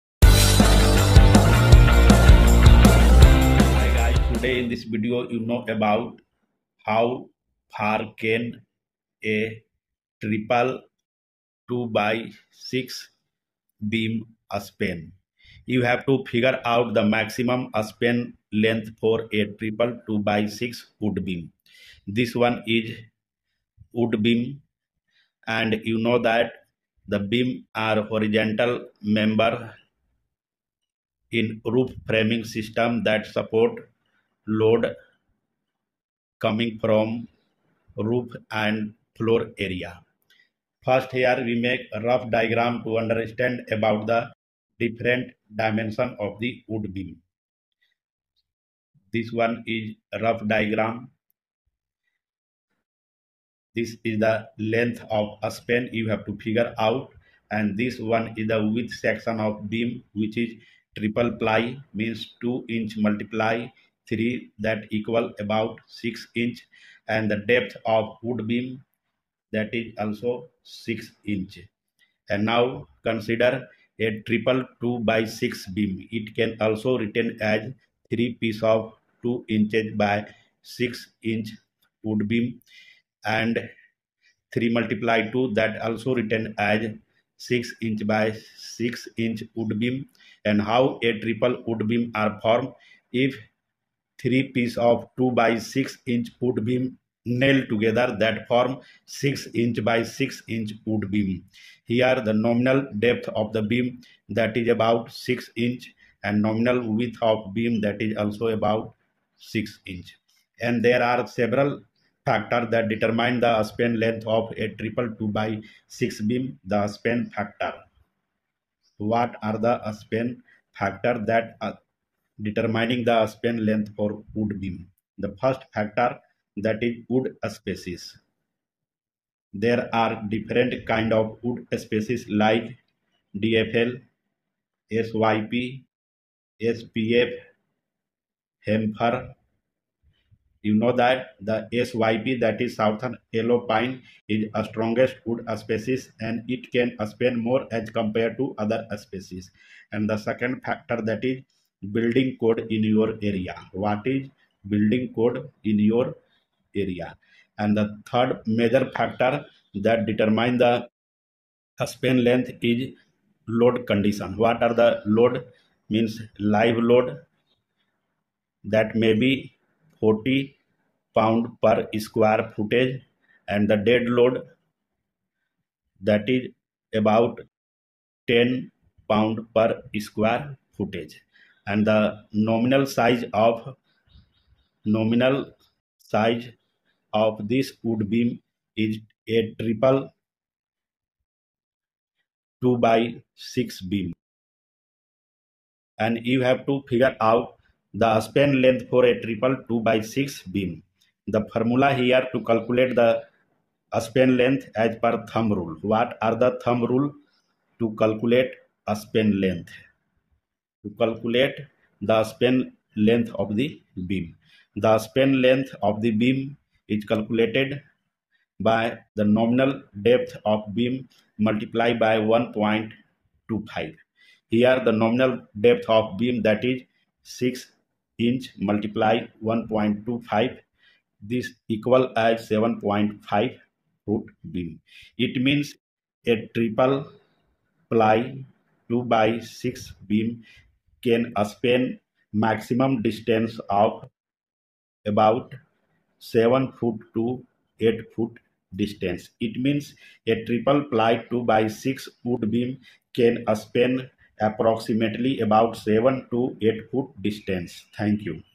A Triple 2x6 Beam Span | How far can a triple 2x6 beam span
The span of a triple 2x6 beam depends on various factors, including the load it needs to support, the type of wood, and whether the load is uniformly distributed or concentrated. Generally, in residential construction, the span is determined by building codes and engineering tables that consider these factors.

For a typical triple 2x6 beam (three 2x6s nailed together), here are some general guidelines for spans under different loading conditions:

1) Floor Loads (40 psf live load + 10 psf dead load): A triple 2x6 beam can span approximately 8 to 9 feet.

2) Roof Loads (20 psf live load + 10 psf dead load): A triple 2x6 beam can span approximately 10 to 12 feet.

3) Deck Loads (60 psf live load): A triple 2x6 beam can span approximately 6 to 8 feet.

These values are approximate and can vary based on specific conditions such as wood species, grade, and environmental factors.